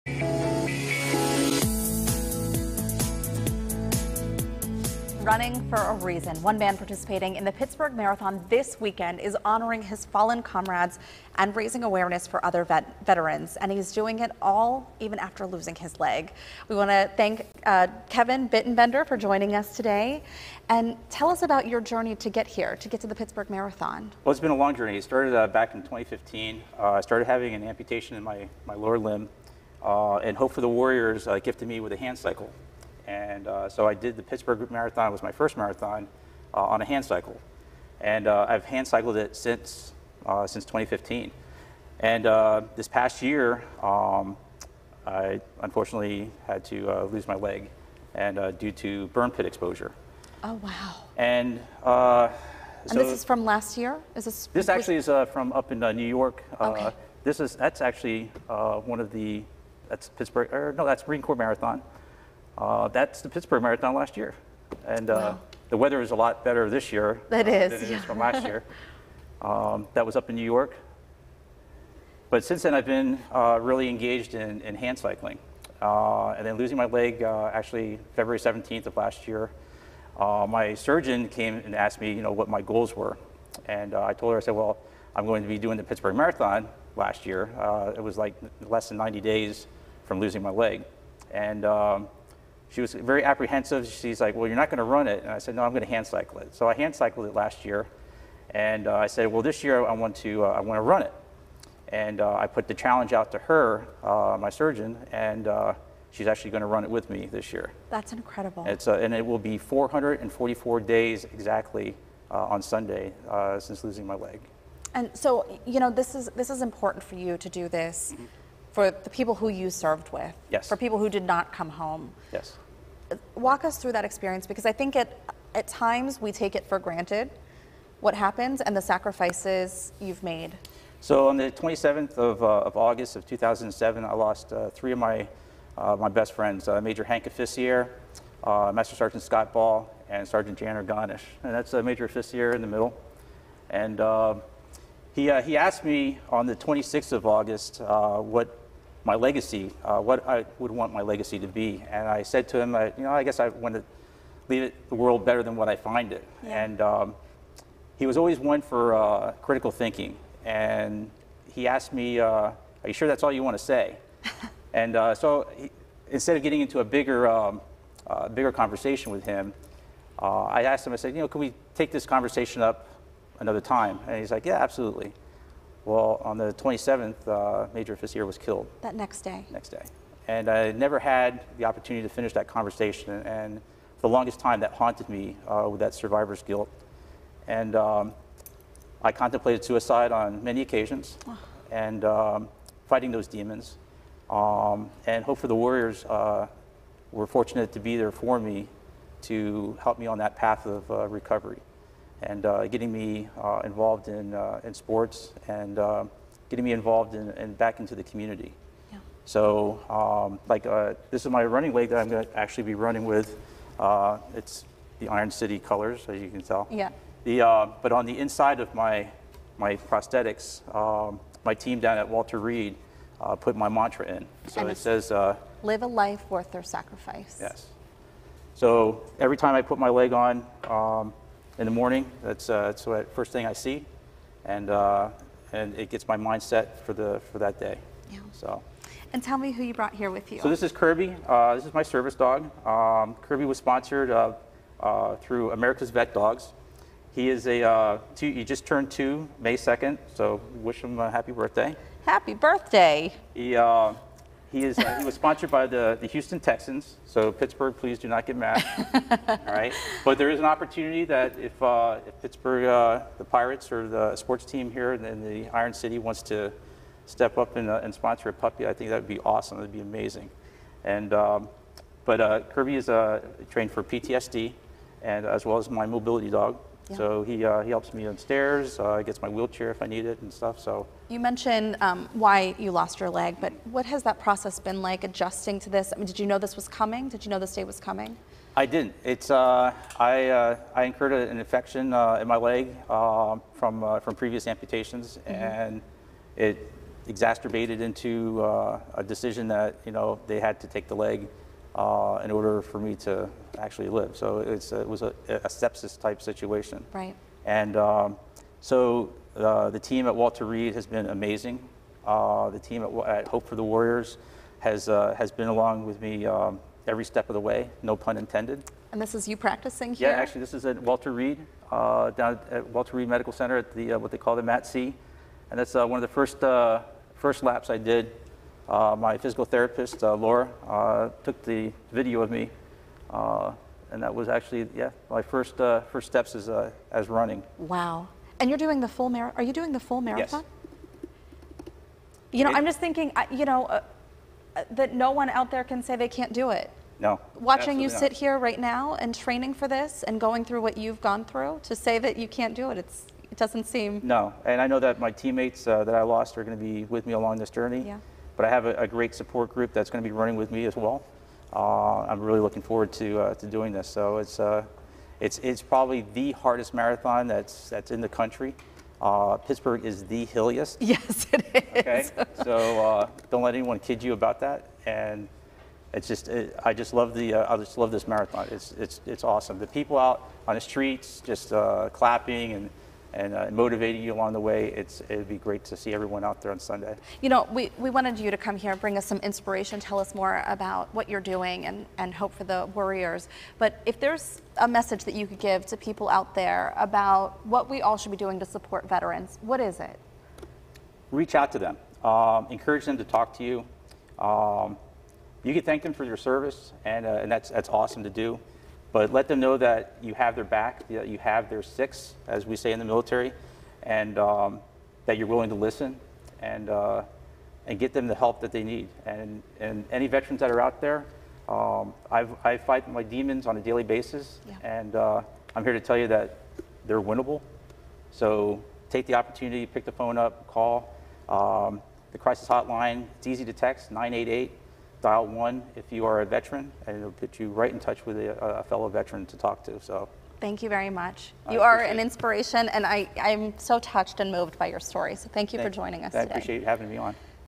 Veteran taking on Pittsburgh Marathon to raise awareness and support fellow warriors
Host Heather Abraham is chatting with Kevin Bittenbender, a veteran, who is taking part in the Pittsburgh Marathon this weekend to raise awareness and support Team Hope for the Warriors.